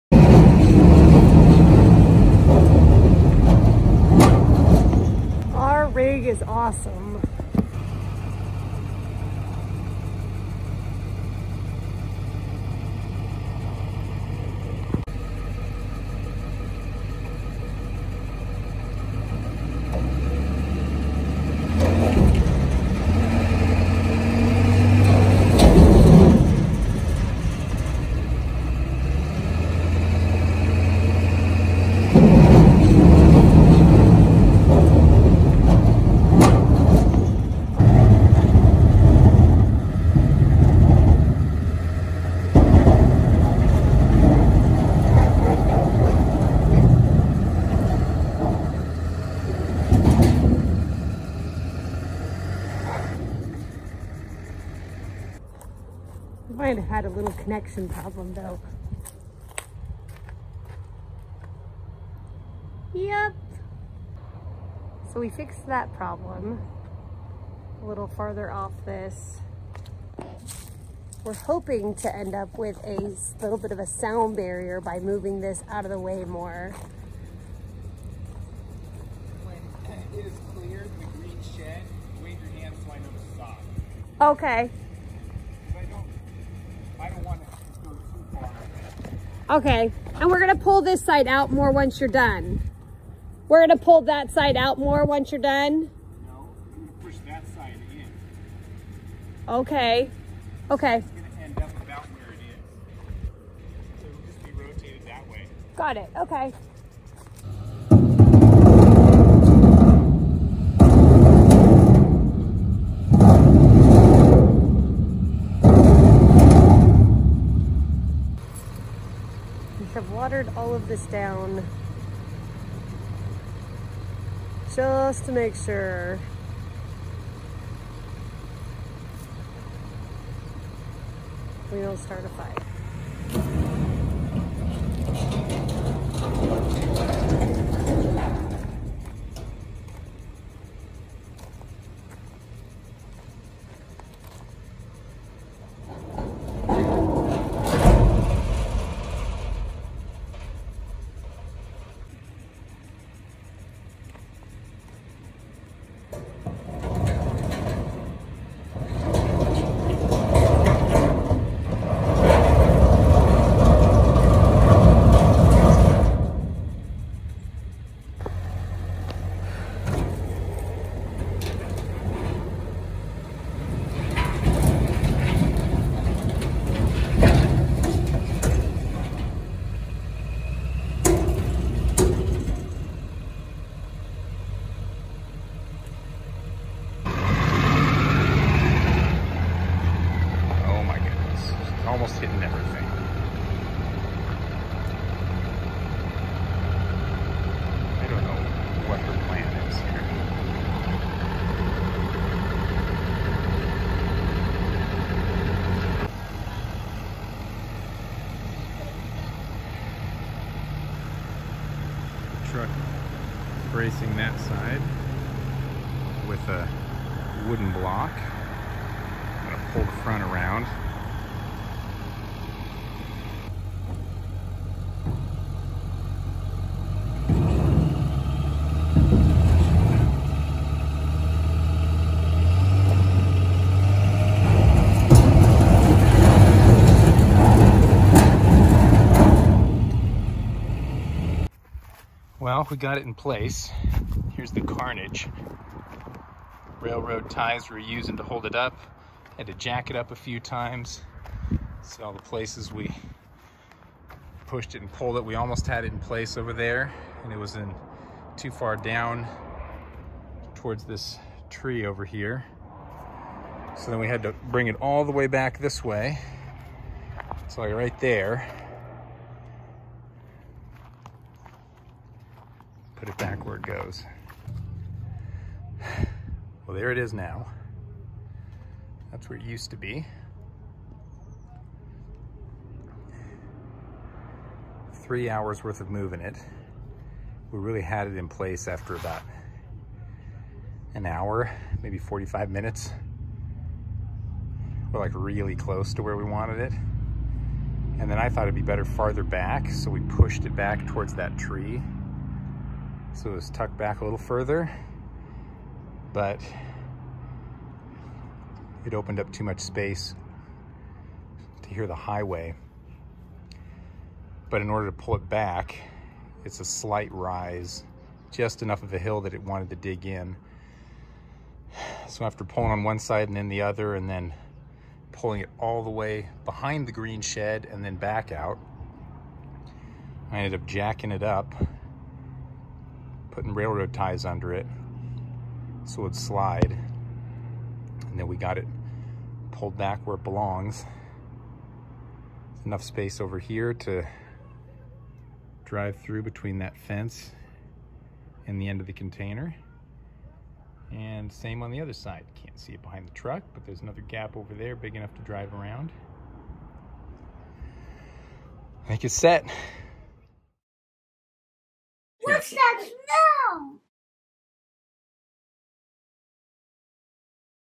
Moving a 40' high cube shipping container with a 7.3L Excursion
Our 2002 Ford Excursion with a 7.3L diesel engine gets it done. We call it "The Unicorn." Put it in 4 wheel drive, low range, chain it up to the container and drive.